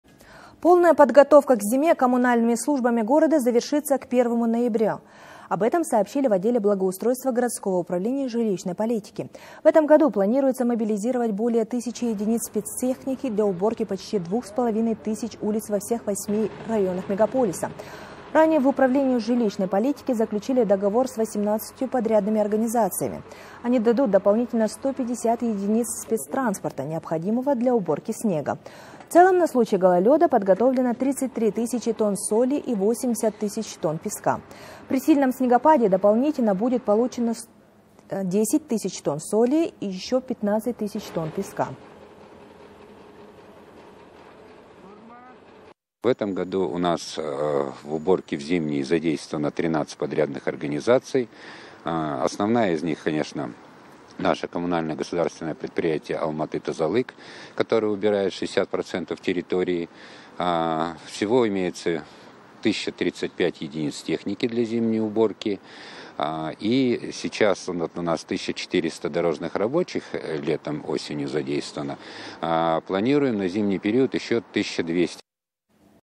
Зима в Алматы: коммунальные службы завершат подготовку к 1 ноября (07.10.20)
Полная подготовка к зиме коммунальными службами города завершится к 1 ноября. Об этом сообщили в отделе благоустройства городского Управления жилищной политики, передает Almaty.tv.